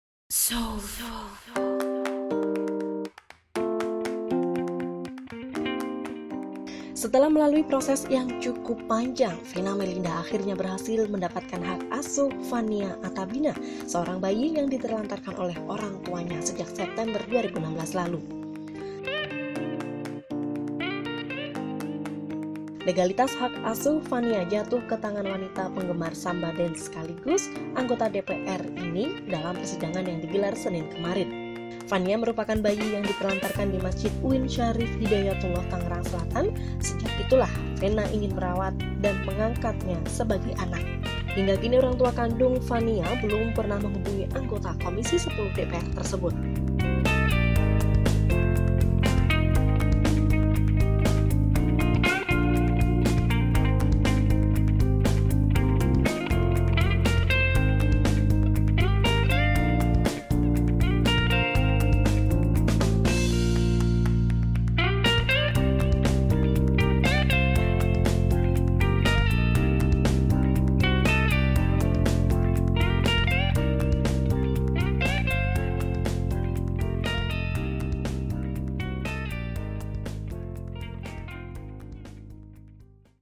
Venna Melinda Resmi Jadi Ibu Bayi Telantar Vania Athabina
Setelah perjuangan panjang, Venna Melinda akhirnya mendapatkan hak asuh Vania Athabina, bayi yang ditelantarkan orang tuanya di Tangerang, September lalu.

Majelis Hakim Pengadilan Agama (PA) Jakarta Selatan mengabulkan legalitas hak asuh Vania kepada Venna dalam persidangan yang digelar kemarin, Senin (10/7).

Sayangnya, pada sidang putusan, anggota Komisi X DPR itu tidak dapat hadir. ’’Yang jelas, hari ini diputuskan bahwa permohonan anak sudah disahkan PA Jakarta Selatan kepada Ibu (Venna, Red),’’ kata Novio Manurung, kuasa hukum Venna.

Vania adalah bayi yang ditelantarkan di masjid UIN Syarif Hidayatullah, Tangerang Selatan, pada September tahun lalu. Sejak itu, Venna merawatnya dan ingin mengangkatnya sebagai anak. Hingga kini, orang tua kandung Vania belum pernah menghubungi Venna.

Meski kini Vania sudah sah menjadi putrinya, mantan bintang sinetron tersebut menegaskan tidak akan memutus silaturahmi Vania dengan orang tua kandungnya.

’’Kapan pun orang tua asli mau ketemu Vania, Bu Venna membuka diri,’’ ungkap Vio. (glo/c14/na)